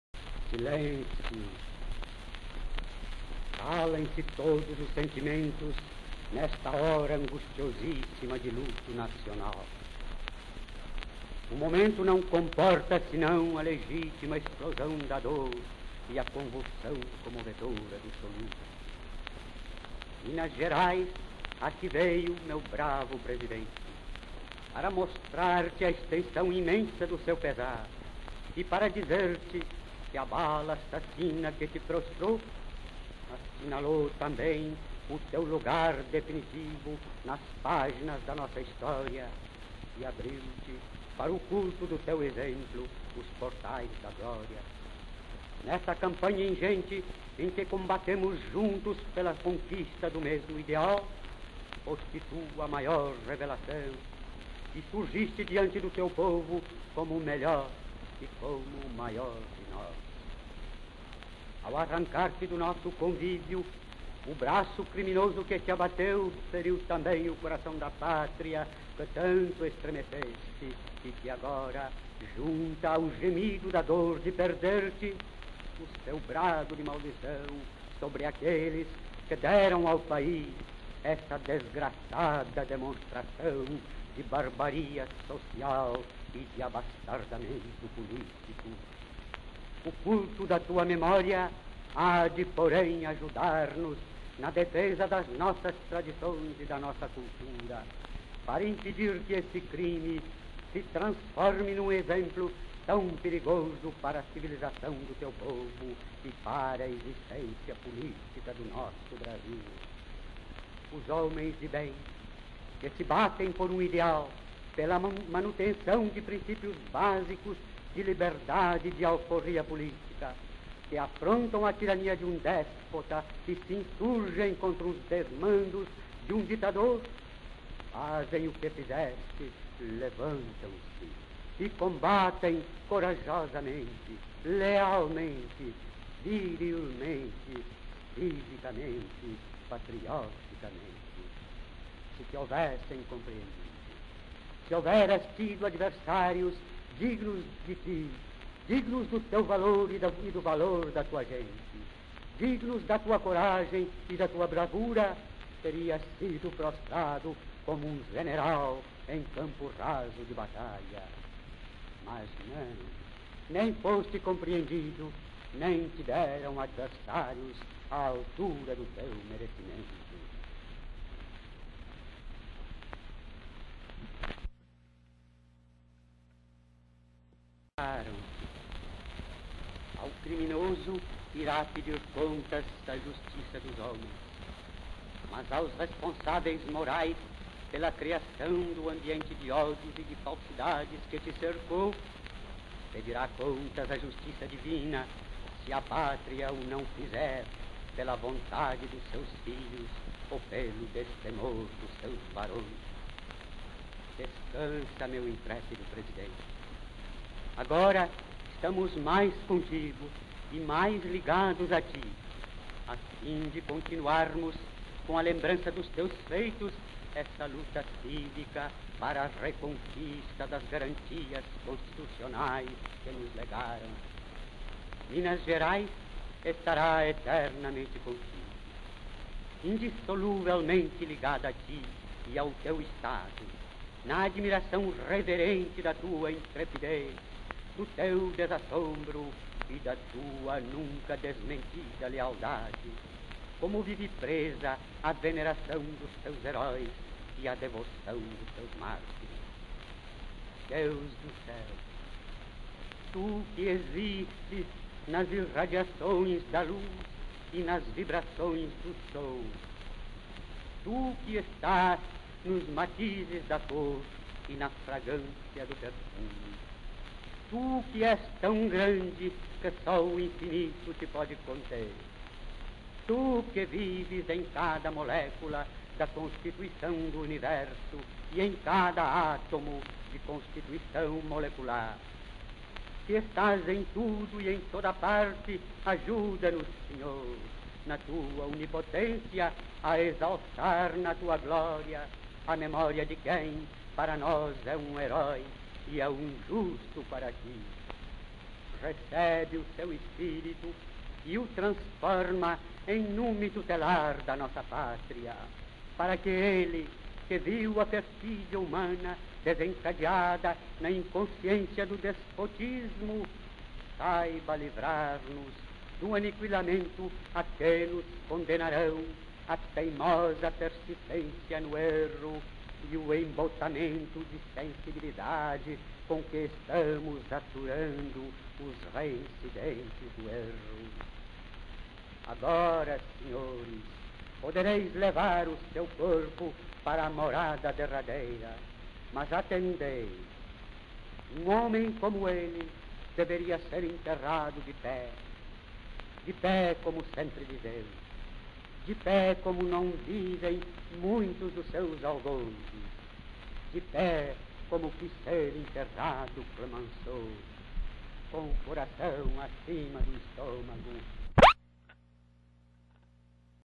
ORAÇÃO A JOÃO PESSOA - - - Discurso do Deputado Pinheiro Chagas (MG) 1930
Oração à João Pessoa.

Discurso proferido pelo Deputado Pinheiro Chagas (MG)    , por ocasião da cerimõnia fúnebre do Presidente da Paraíba e candidato a vice-presidente do Brasil na chapa de Getúlio Vargas.
Nas eleições de março de 1930  é eleito Júlio Prestes (SP) , mas a oposição alega que houve fraude.  Há um clima de incerteza política e no dia 26 de julho de 1930 , João Pessoaa é assassinado num crime ocorrido numa confeitaria do Recife. As razões do homicídio são passionais, porém a oposição ao Governo de Washington Luiz  irá fazer de João Pessoa , um mártir da Revolução de 30 que levará Getúlio Vargas ao Poder.
O discurso  aqui disponível foi pronunciado pelo Deputado Federal  Pinheiro  Chagas, representante de Minas Gerais  no dia 7 de agosto de 1930 e foi gravado em Discos Odeon  10714 A/B . No final do discurso ele cita o nome de Georges Clemenceau, que foi Primeiro Ministro da França durante a Primeira Guerra Mundial (1914/1918) e faleceu em 1929. Clemenceau pediu para ser enterrado em pé , da mesma maneira que os soldados franceses mortos na guerra, e Pinheiro Chagas  faz referencia á este fato.